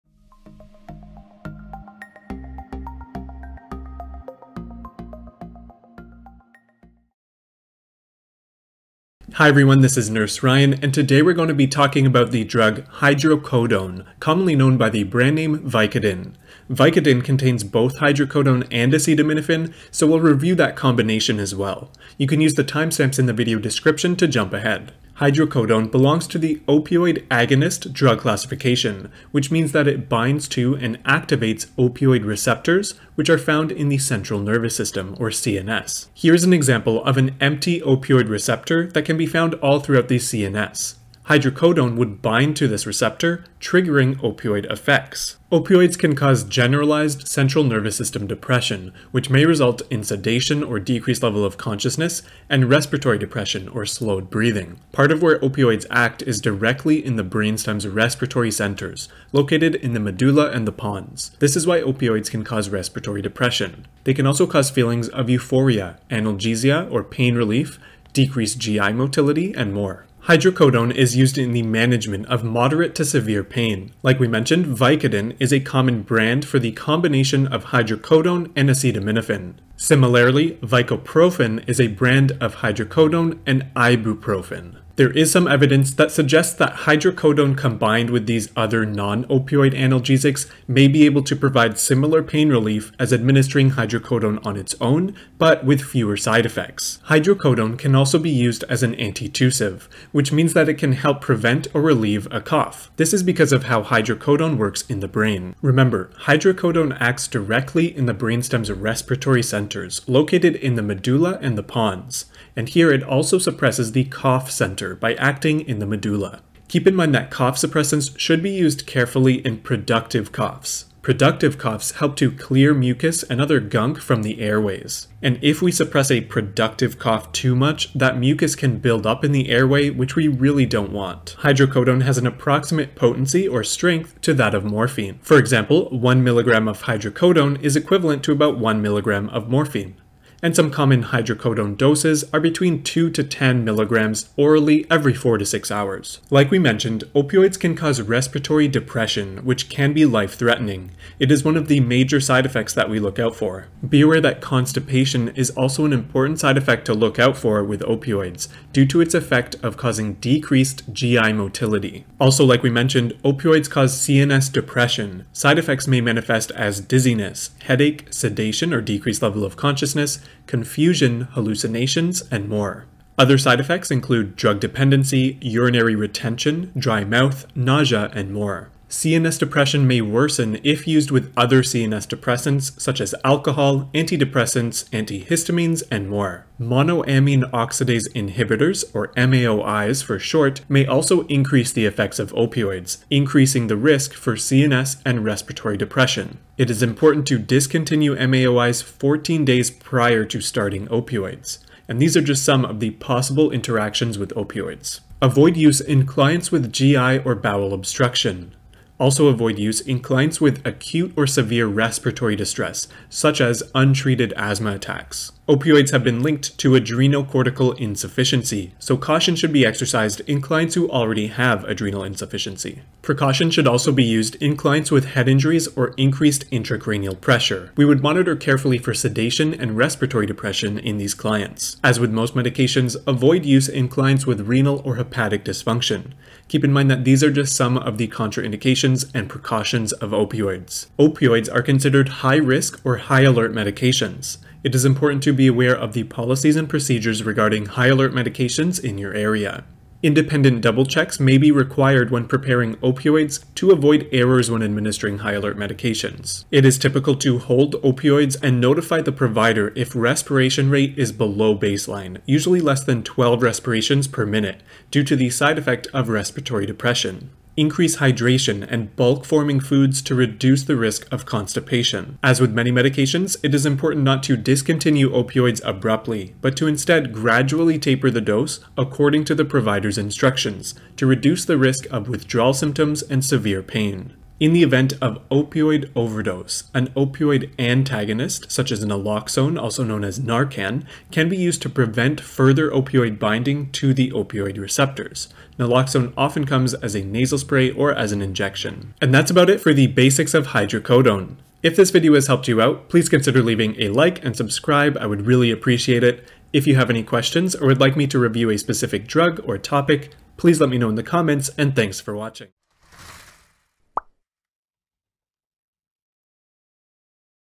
Hydrocodone (Vicodin) Nursing Drug Card (Simplified) - Pharmacology
NCLEX Review: Hydrocodone (Vicodin, Vicoprofen)  - Mechanism of Action, Uses, Side effects, and Nursing considerations

Action: 0:25
Uses: 1:10
Strength/Dosing: 2:11
Side effects: 2:26
Interactions: 2:59
Contraindications: 3:25
Nursing considerations: 4:00

Correction to increasing bulk-forming foods for constipation:
"Increasing dietary fiber, fluid intake, and physical exercise are the backbone to prevention therapy... All types of laxatives can be used as initial therapy except for the bulk-forming laxatives... The only laxatives that should be avoided are the bulk-forming laxatives, like psyllium. These laxatives increase the bulk of the stools, distend the colon, and augment peristalsis. Opioids prevent peristalsis of the increased bulk which worsens abdominal pain and can contribute to bowel obstruction." - Opioid Induced Constipation (2021) - PMID: 29630236

"When people think of opioid use problems or addictions, they may think of street-obtained, or 'street' opioids, such as heroin or fentanyl. However, problematic opioid use can also include improperly taking prescribed opioid medications such as oxycodone, morphine or codeine, or taking an opioid medication that was not prescribed for you.

Opioids are a class of powerful drugs that are usually prescribed to treat severe pain. If opioids are abused, they can create feelings of intense pleasure or euphoria, but they can also lead to fatal overdose. Opioids are an effective medication when used as prescribed, but they carry a risk of addiction because of their powerful effects.

If someone is overdosing, call 911 right away! You can then use your naloxone kit while you wait for medical help." - camh.ca - Opioid Addiction (Retrieved Oct 21/2021)

Brainstem image: Wikipedia - Brainstem

Narcan image: Narcan.com

Intro: adMJ707
Outro: adMJ707